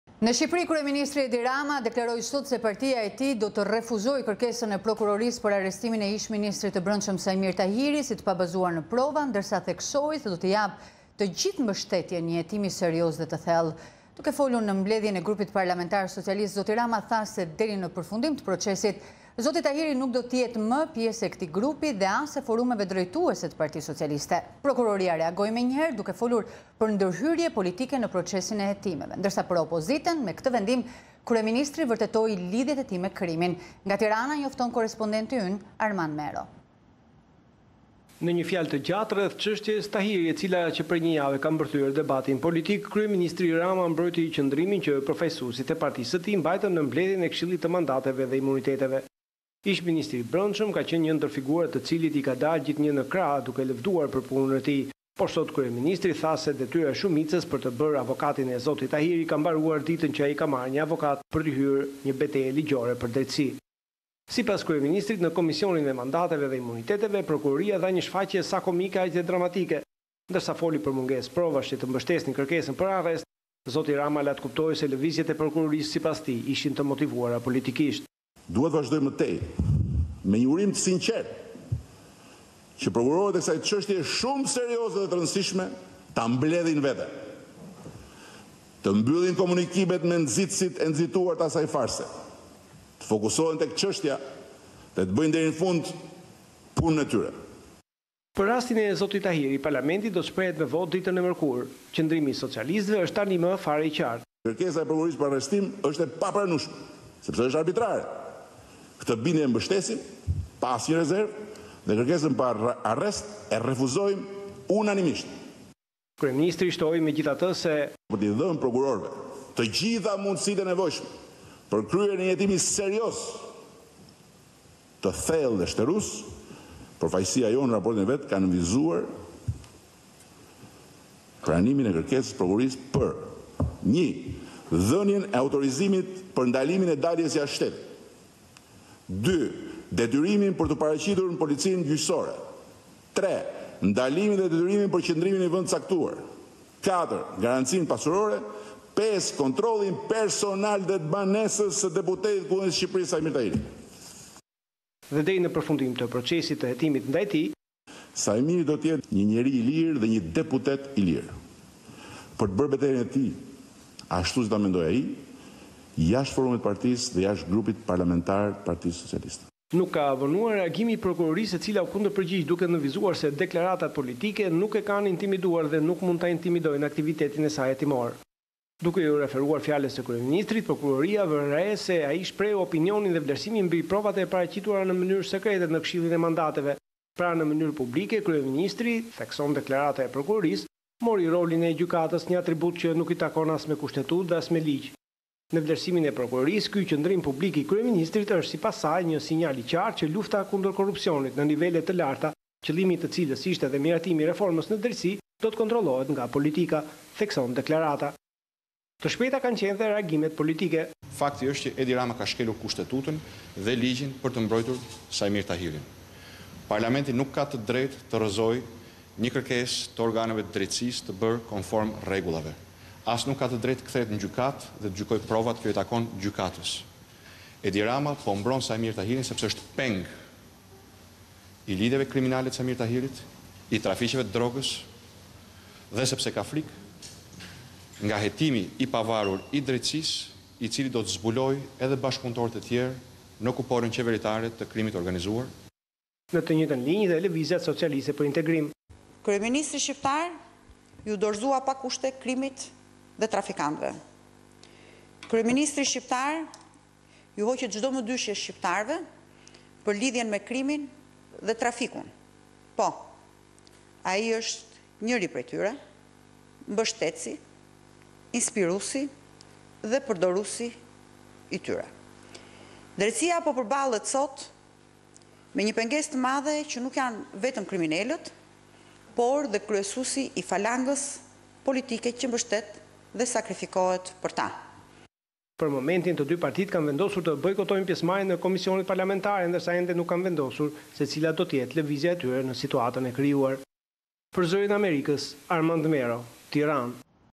Rama: Jo arrestimit pa prova të Tahirit
Në Shqipëri kryeministri Edi Rama  deklaroi sot se partia e tij do të refuzojë kërkesën e Prokurorisë për arrestimin e ish ministrit të Brendshëm Saimir Tahiri si “të pabazuar në prova”, ndërsa theksoi se do t’i japë të gjithë mbështeten “nje hetimi serioz e të thellë”.